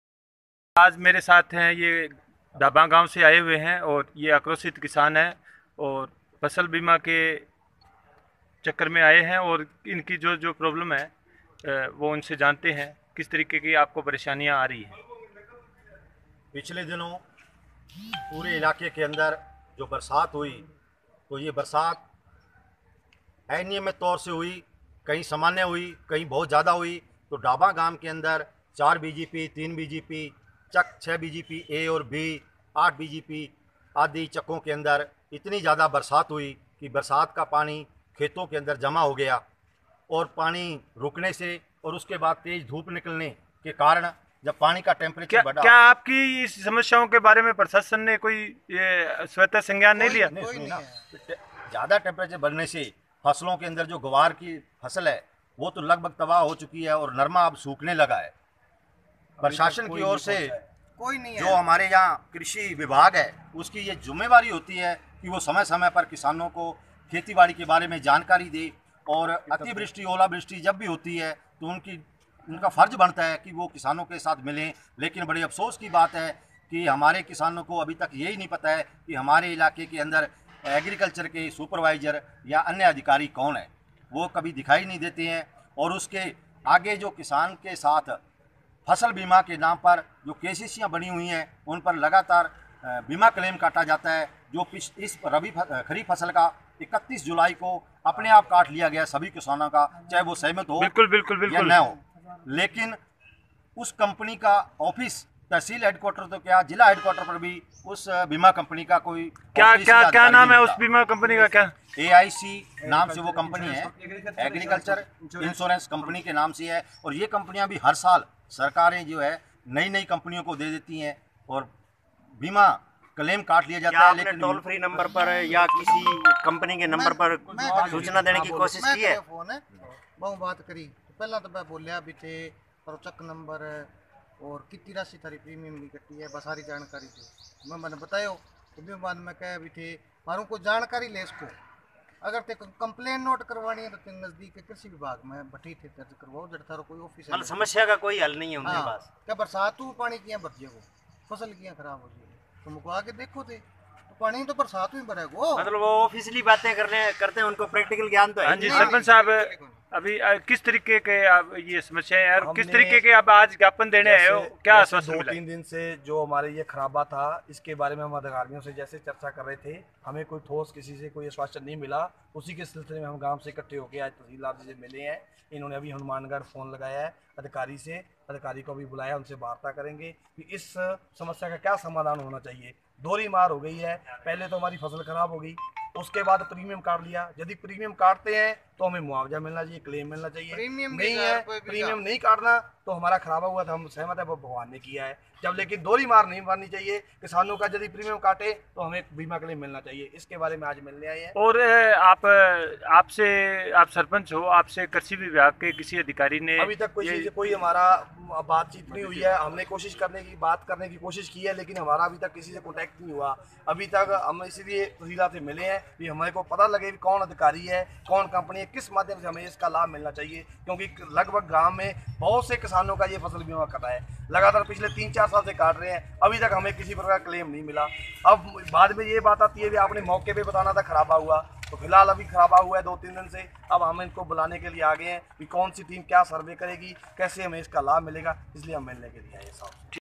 किसान समस्या7 August 2019
आज गांव ढाबा के लोगों ने बरसात के कारण पिछले की हुई खराबी में बीमा कंपनी द्वारा की गई ना सुनवाई के खिलाफ उपखंड अधिकारी संगरिया को ज्ञापन दिया अगर उनकी सुनवाई नहीं हुई तो आंदोलन किया जाएगा